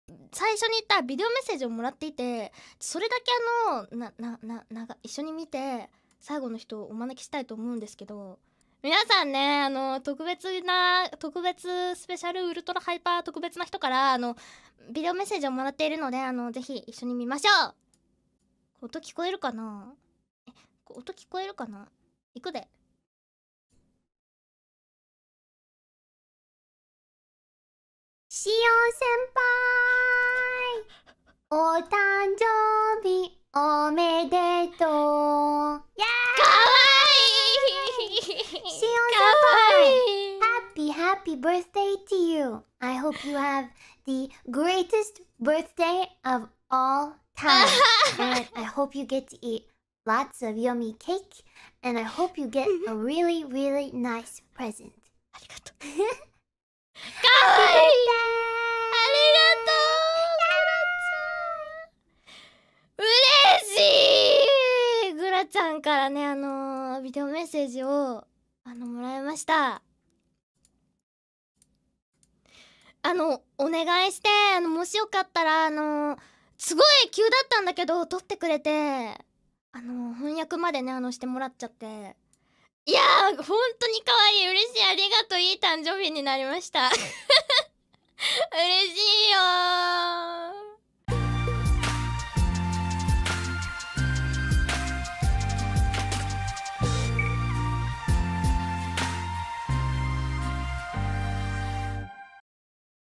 【Hololive】Shion: Birthday Message From Gura【Eng Sub】
Shion got a video message from Gura on her birthday.